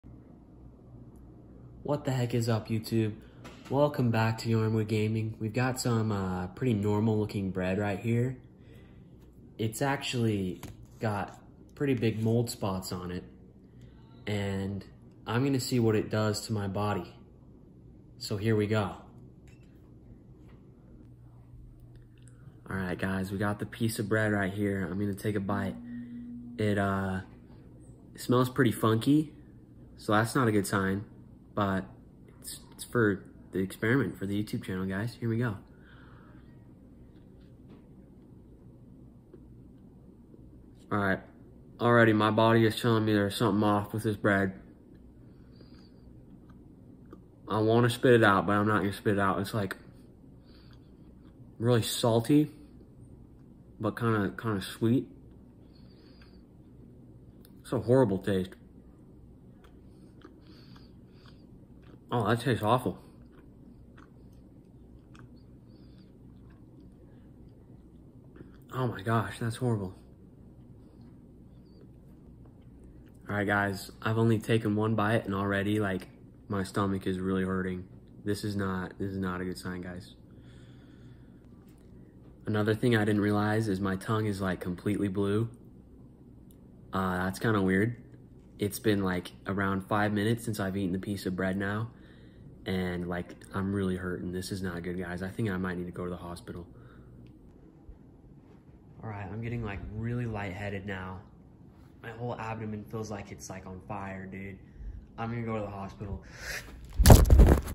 Eating moldy bread!!!
This was completely fake but if this video gets 1,000,000 views in 7 days I will eat real moldy bread…

Bread, moldy, gross, funny.